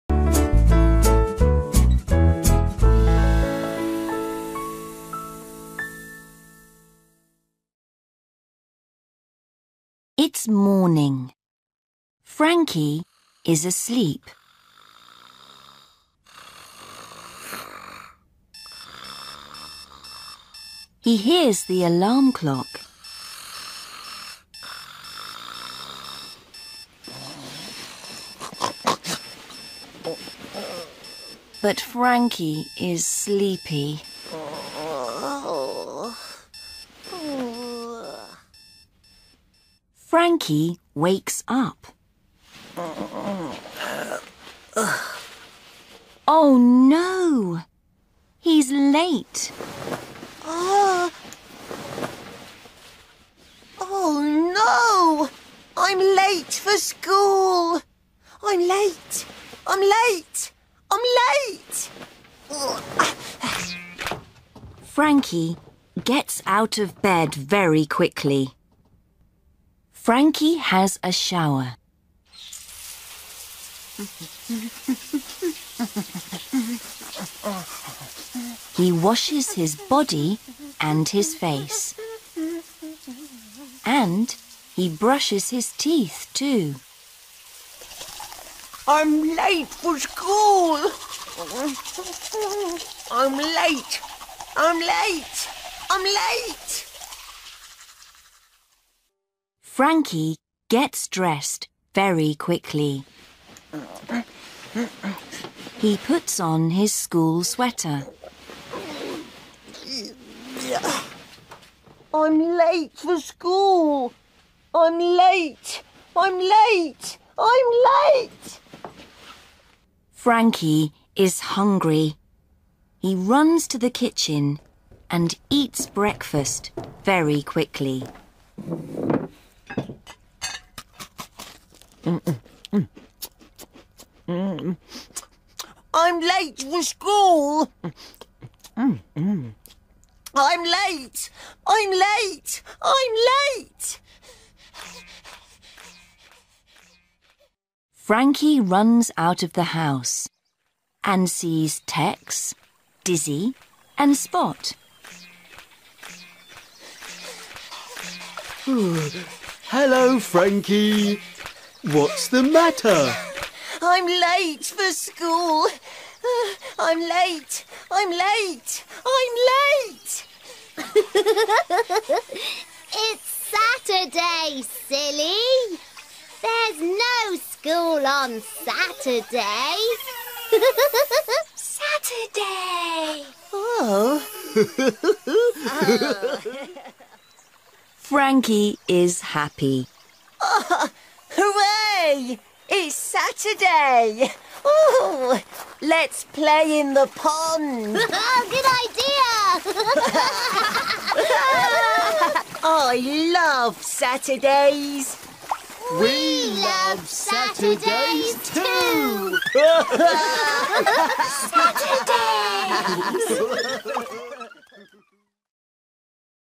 Mirasol School - Kinder- Story: I'm late
Story Time: I'm late

Historia sobre Frankie y su rutina en la mañana para ir a la Escuela.

Todos los derechos pertenecen a Jump In, Oxford University Press.